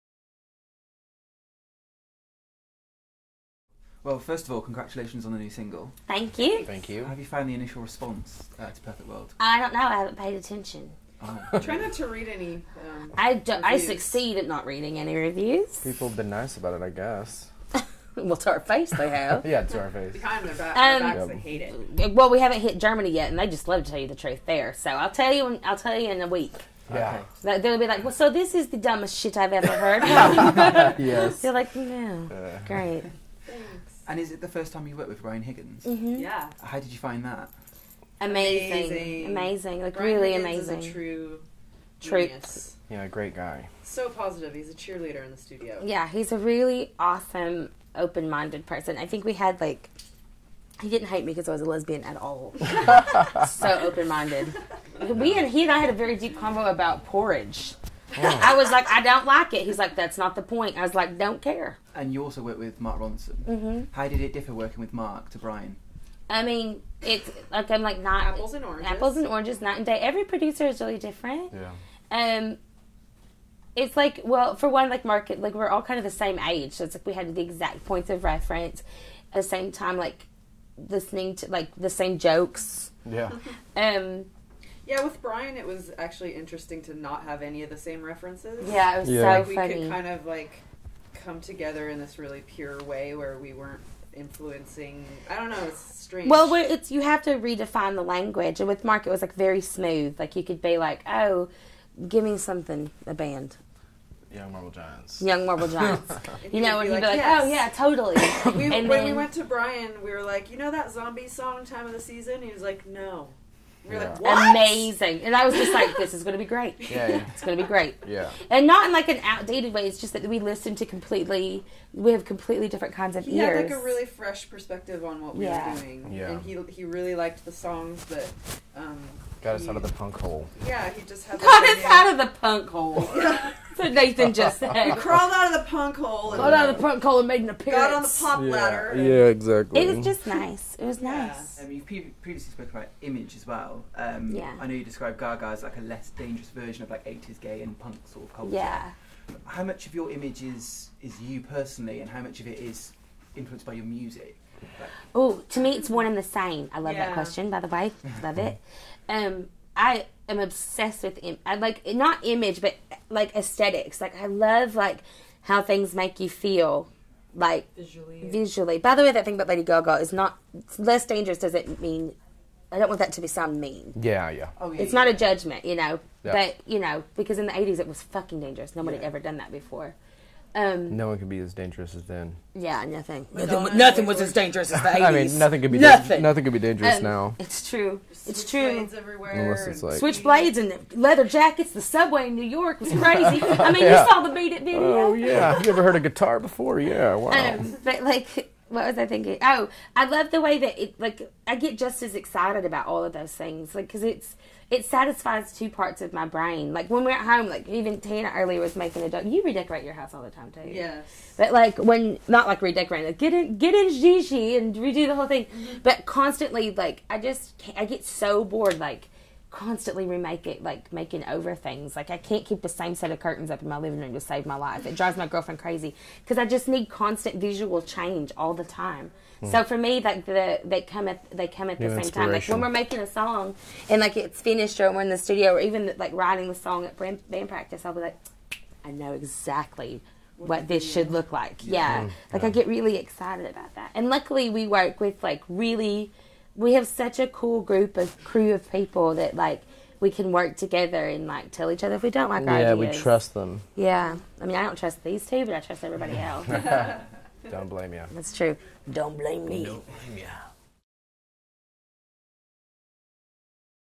Gossip chat new single and album
Digital Spy chats to Gossip about their new material and working with producer Mark Ronson.